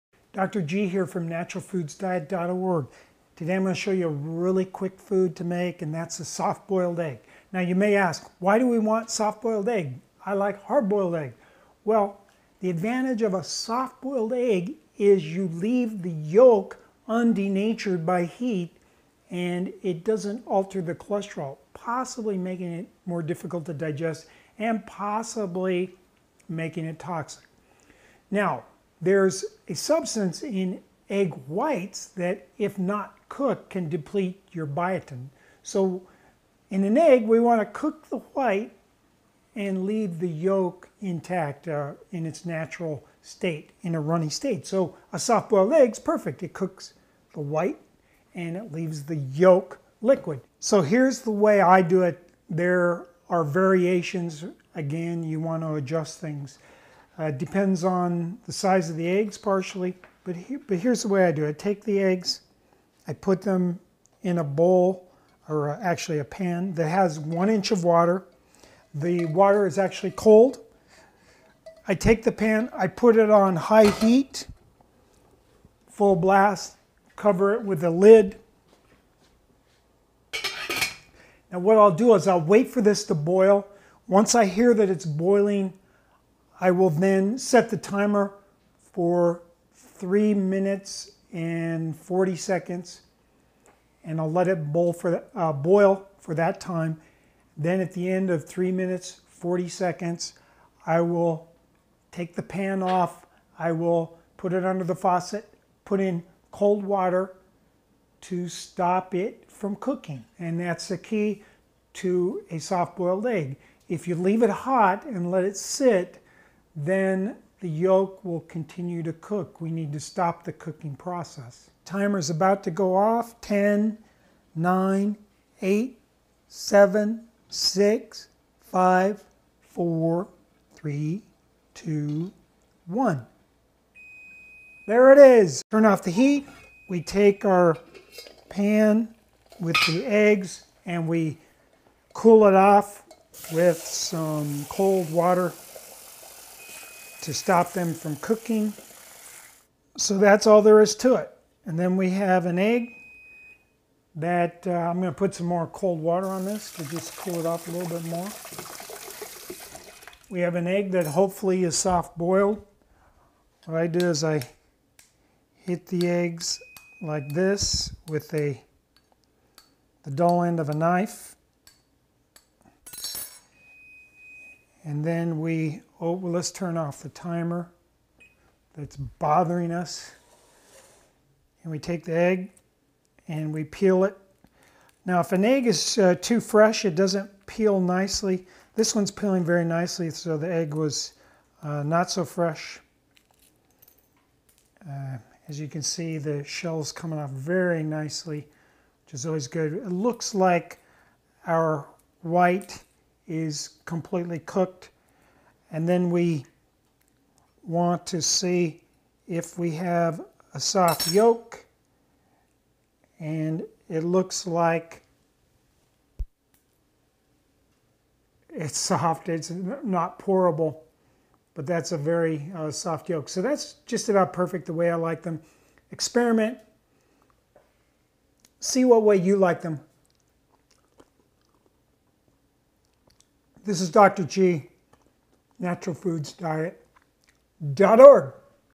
Perfect Hard Boiled Egg Recipe. How To Cook Hard Boiled Eggs
Learn how to cook a perfect hard boiled, or soft boiled, egg in this recipe video with Dr. Richard Gerhauser of Natural Foods Diet.

Google Search: perfect boiled egg, how to boil a egg, boiled egg, eggs